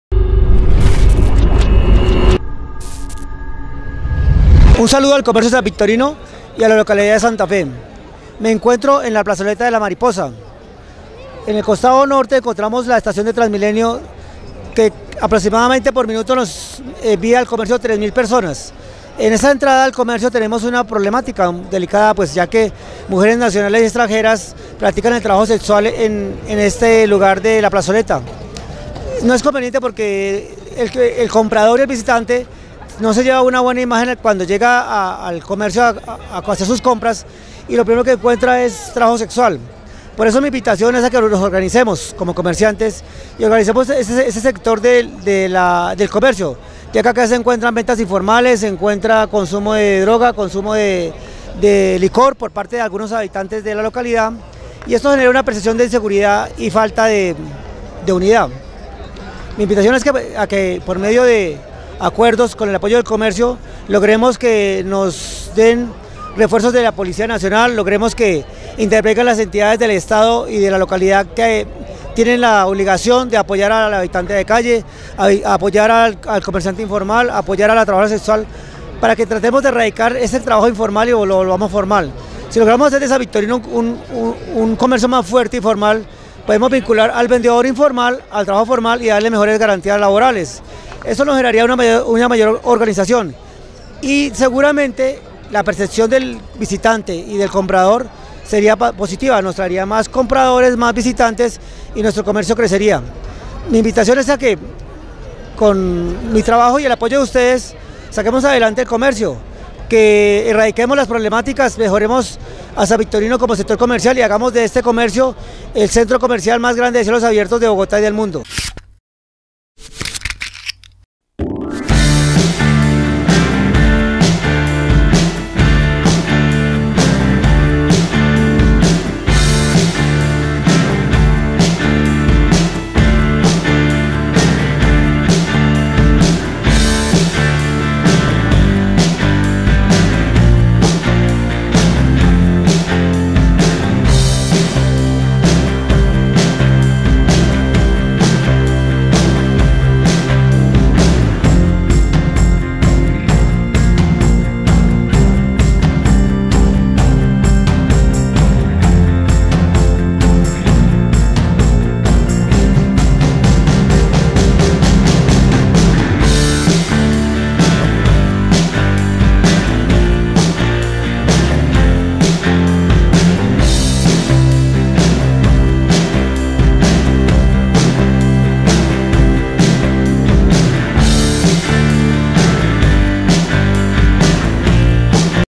Plazoleta de la Mariposa, problemática de Convivencia en San Victorino
Diego Camargo candidato a edil por la localidad de santa fe para el periodo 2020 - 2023, nos presenta la problemática de la Plazoleta de la Mariposa en el sector de San Victorino...